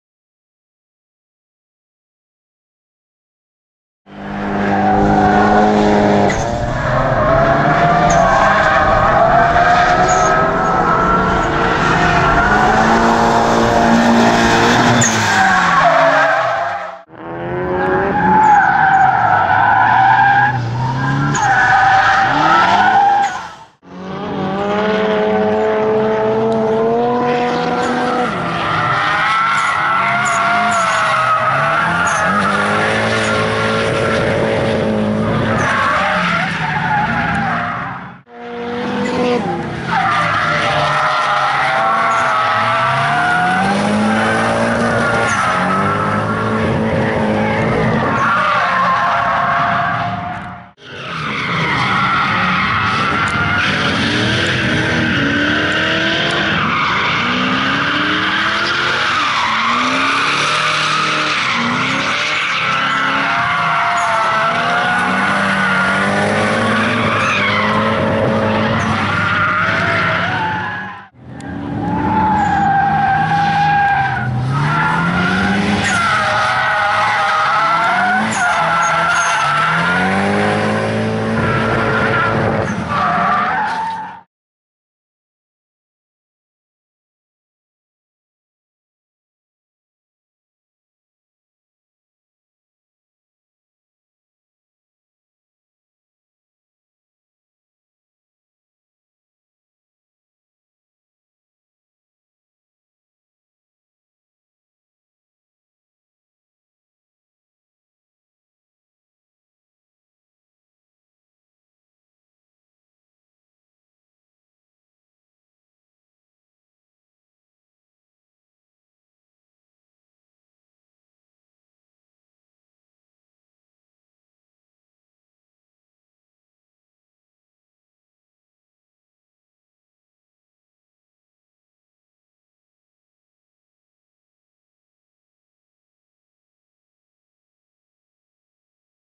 Pat Haire - 6 video clips and some photos
March 4th, 2023, Slyde Factory, Nashville Superspeedway, Season Opener